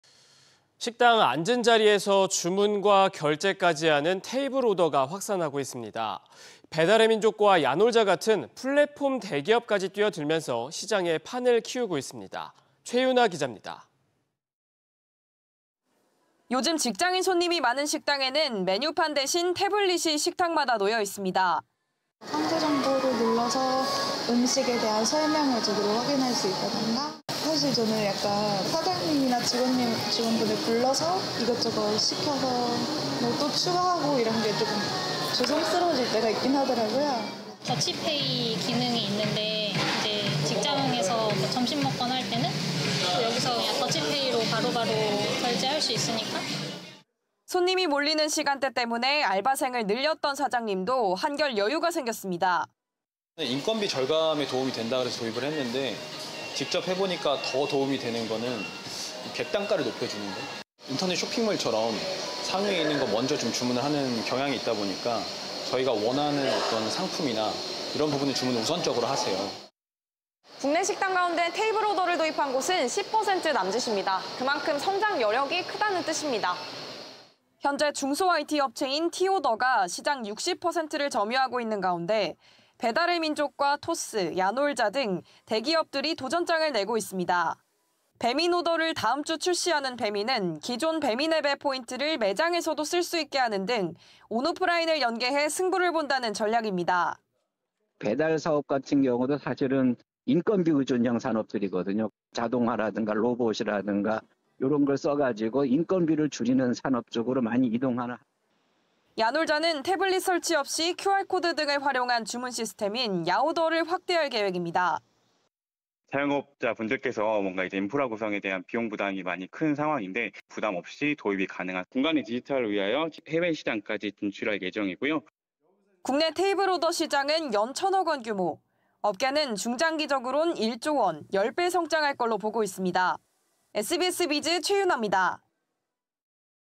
배민·야놀자도 참전…테이블오더 판 키운다
[앵커]

식당 앉은자리에서 주문과 결제까지 하는 테이블오더가 확산하고 있습니다.

배달의민족과 야놀자 같은 플랫폼 대기업까지 뛰어들면서 시장의 판을 키우고 있습니다.

최윤하 기자입니다.